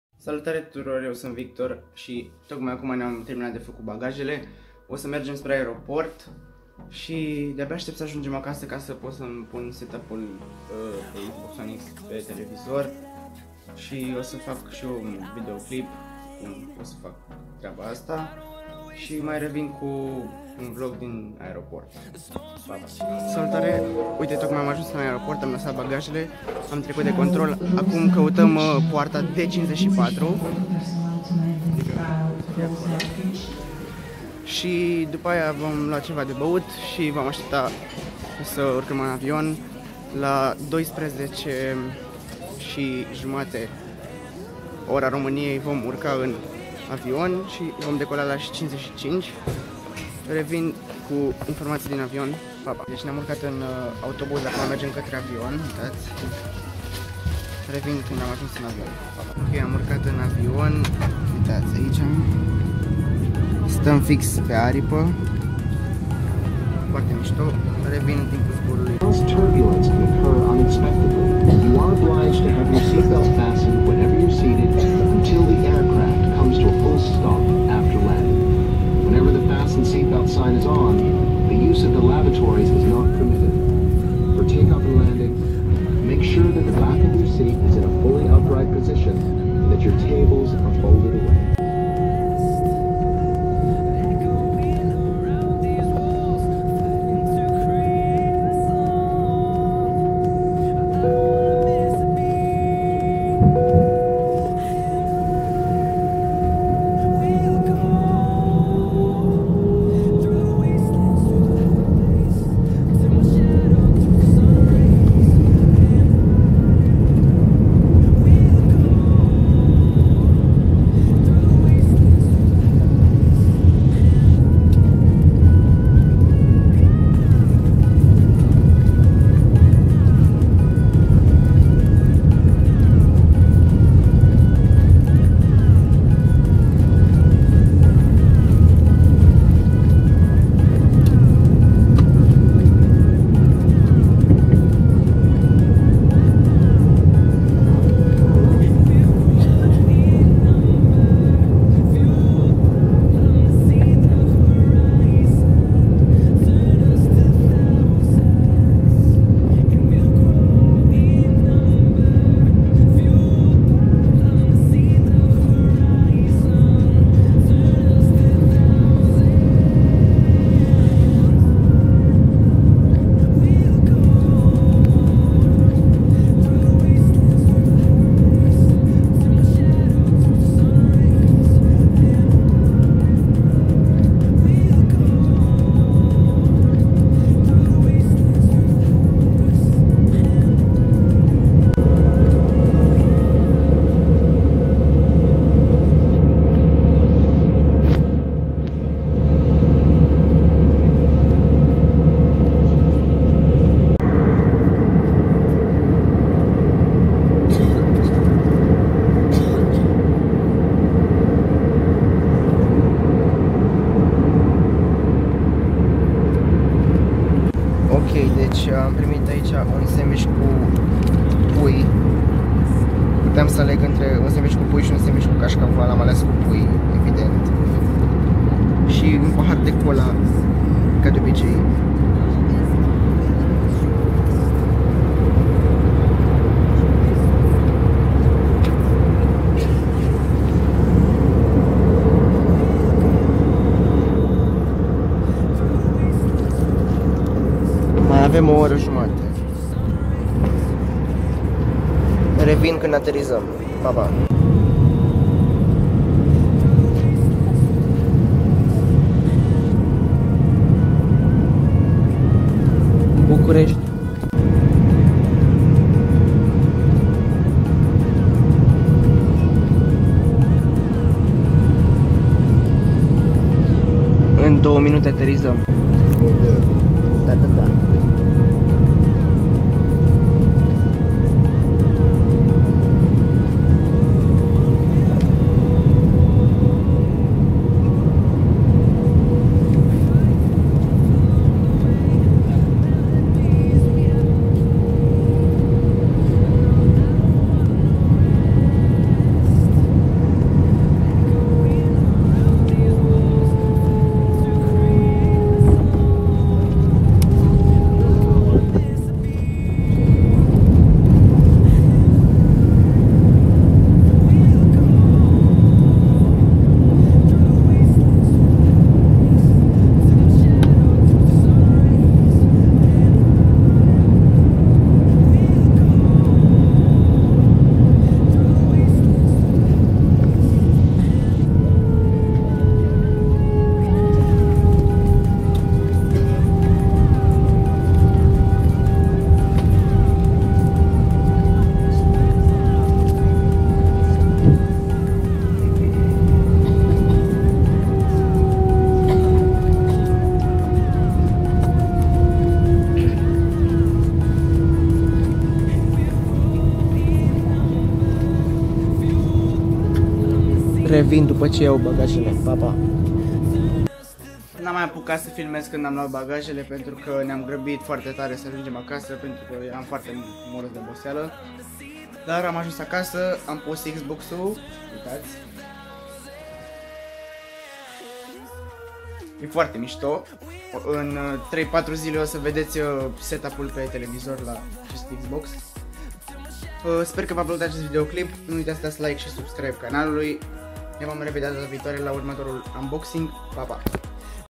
Gata vacanța...(am ajuns acasa)
Misto si vacanta asta.
Voi reveni cu video-uri noi(unboxing-uri si vloguri)